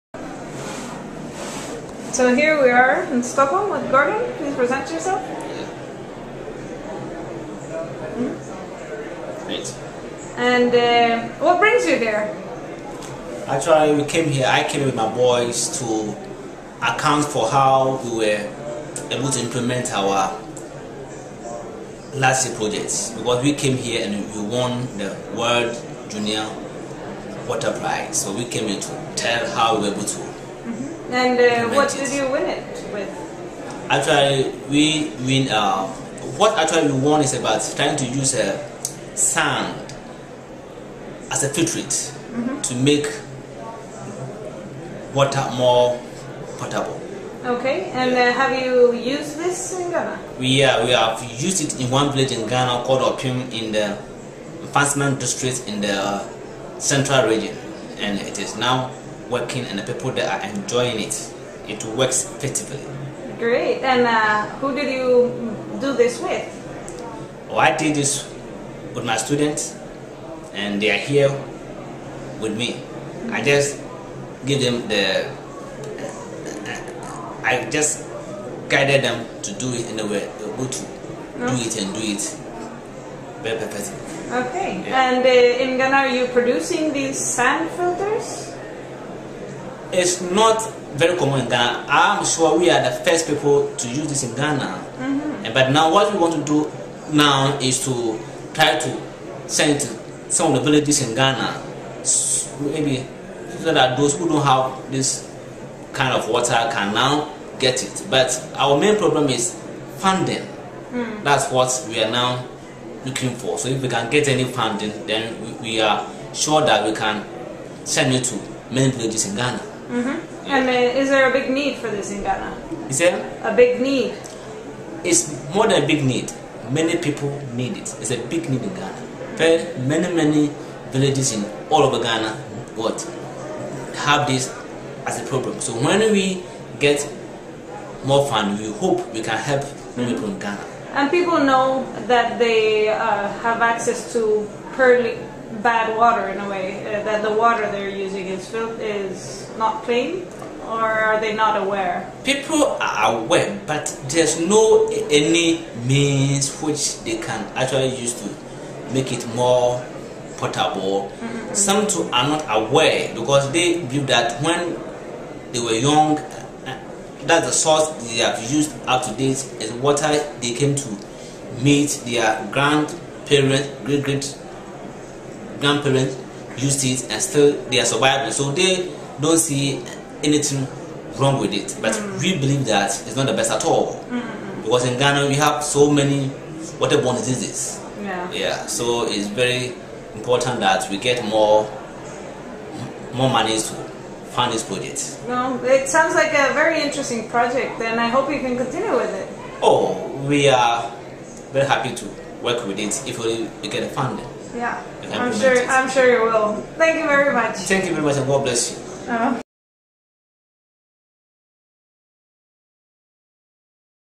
Gordon Sarapong on sand water filter done in Ghana
Interview with Gordon Sarapong, a teacher at Mfantsipin School in Ghana who is here accompanying students from his school that won last years Stockhols Junior's Water Prize with their sand filter for untreated waters. They are implementing this filter for it's use around Ghana, hoping to spread this out if future funding is found. Video shot at the World Water Week 2011 in Stockholm on the 22nd of Agust by Isabel Ordonez. (Aug 23, 2011)